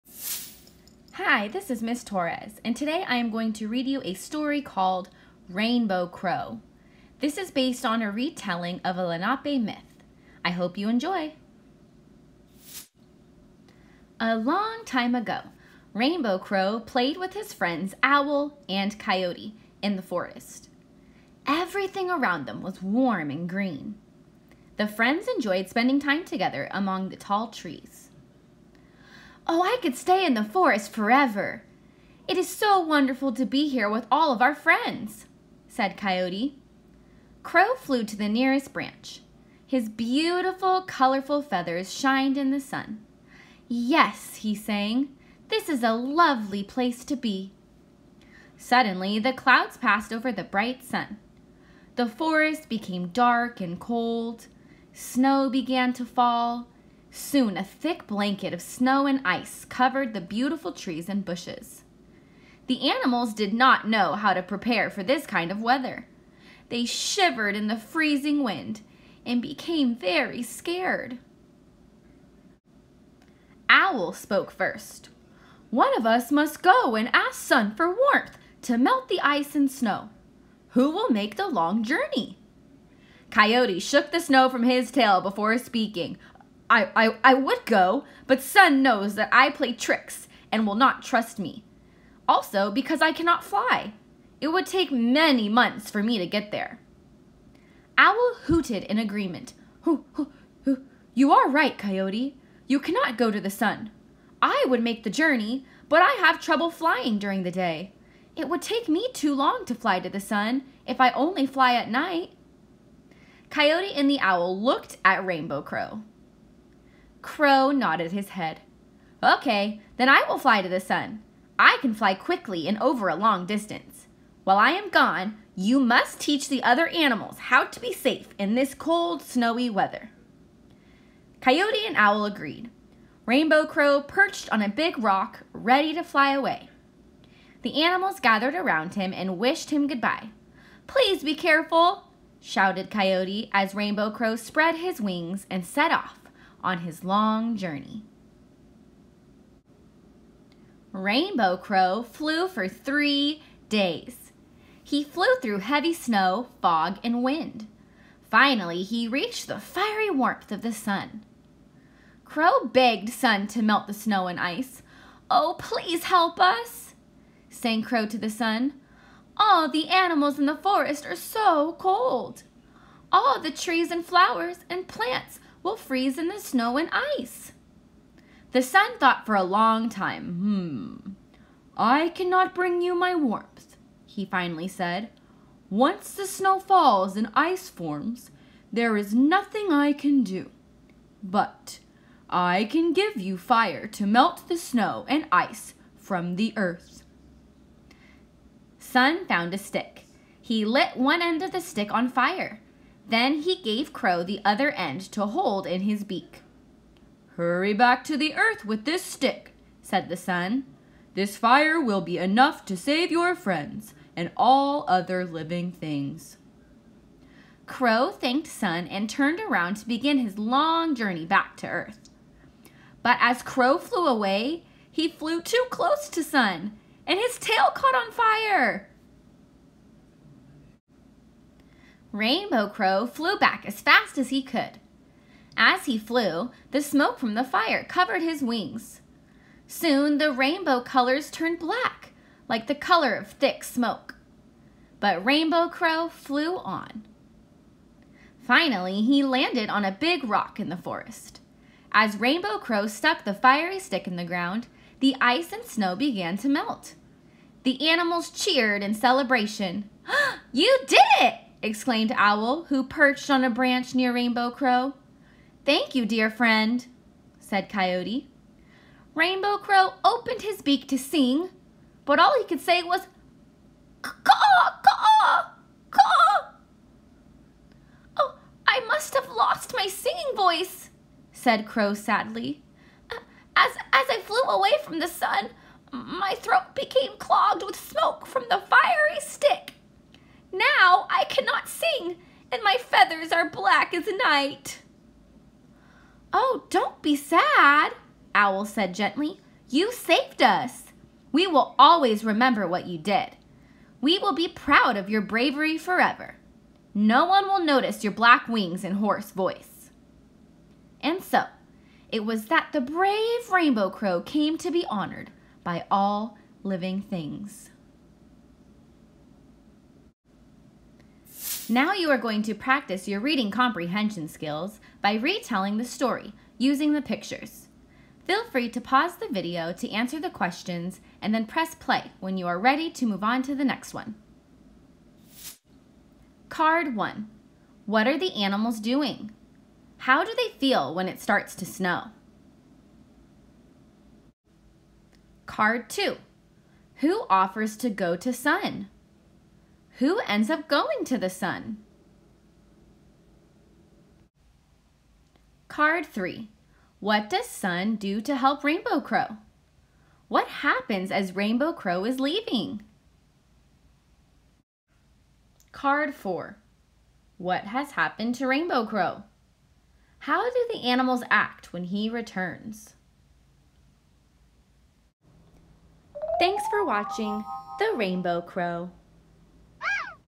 "The Rainbow Crow" Read Aloud by Ms. Torres
Kindergarten Wonders Interactive Read Aloud Unit 6 Week 3